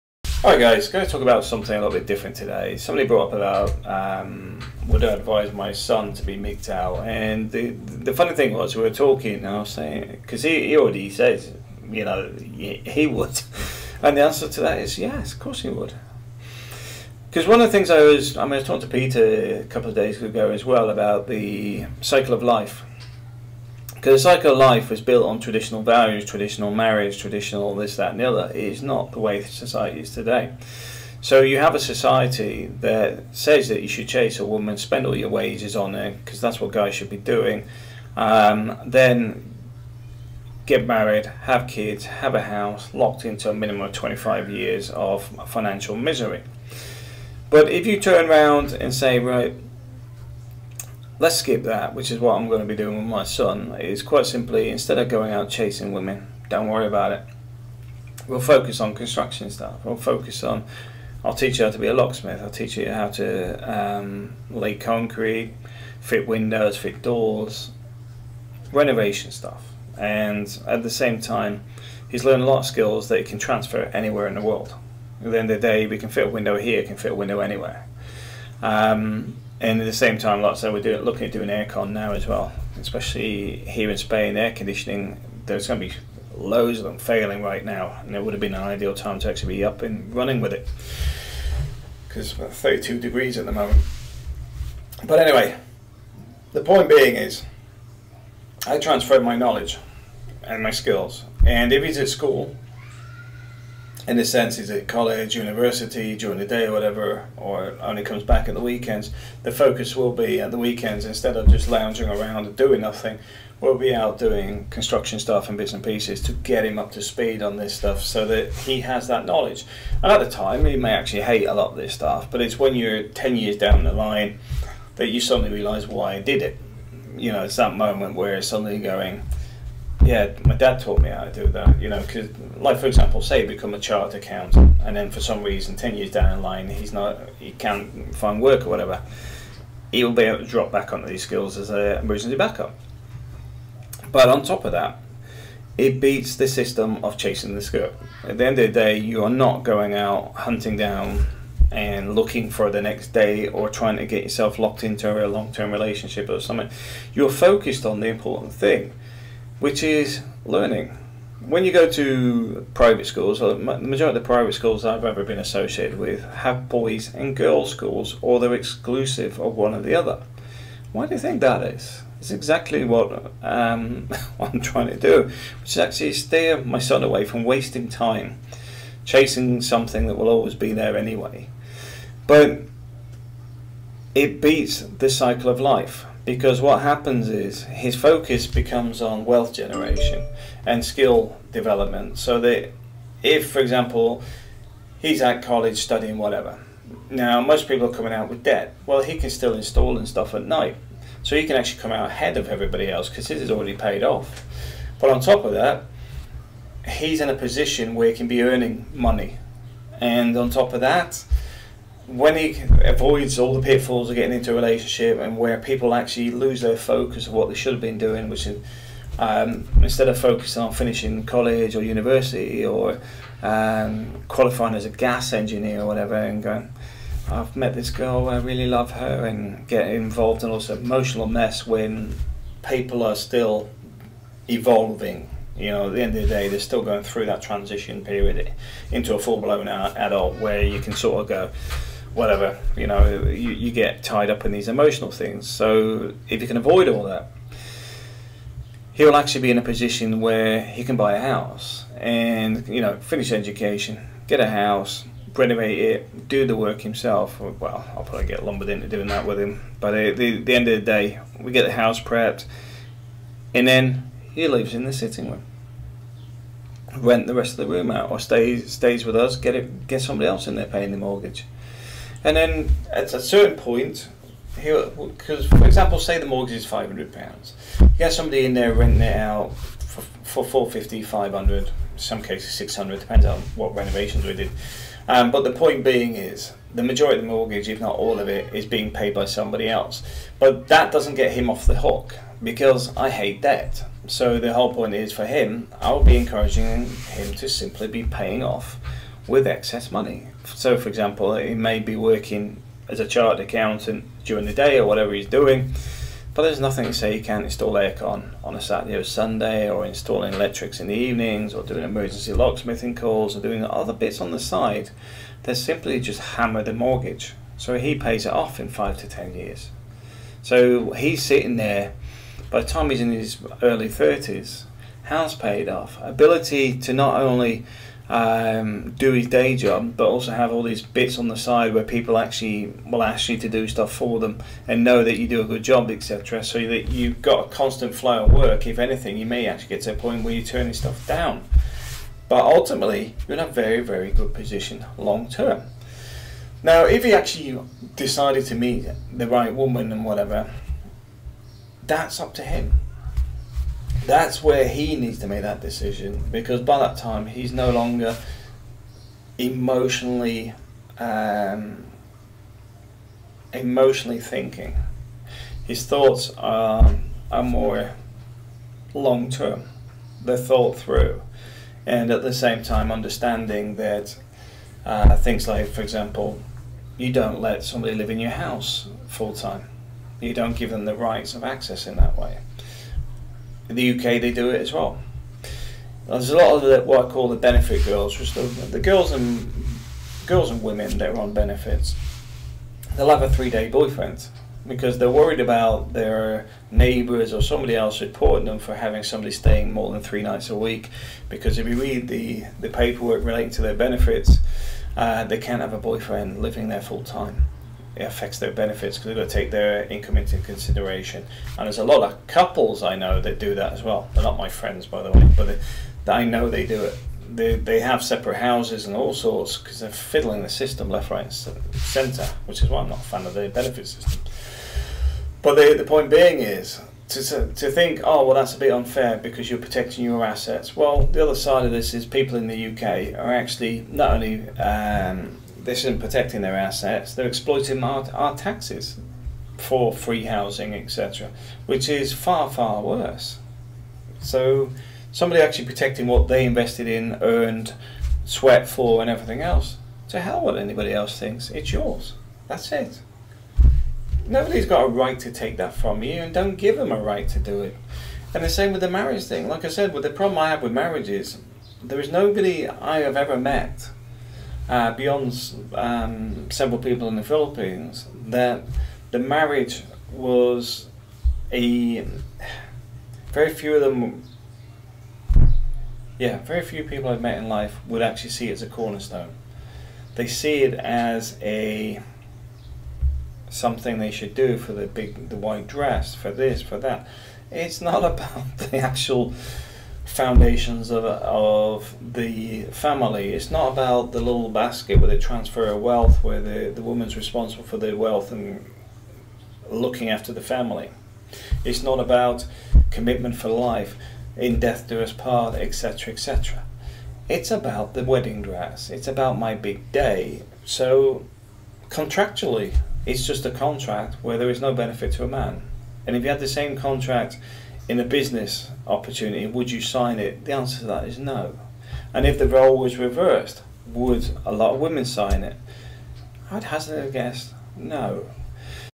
Would you advise your son to be MGTOW?
Would you advise your child to be MGTOW ? I have to admit in today's world that women should also be advising their son's to go down the route of securing their own future and avoiding the pitfalls of marriage.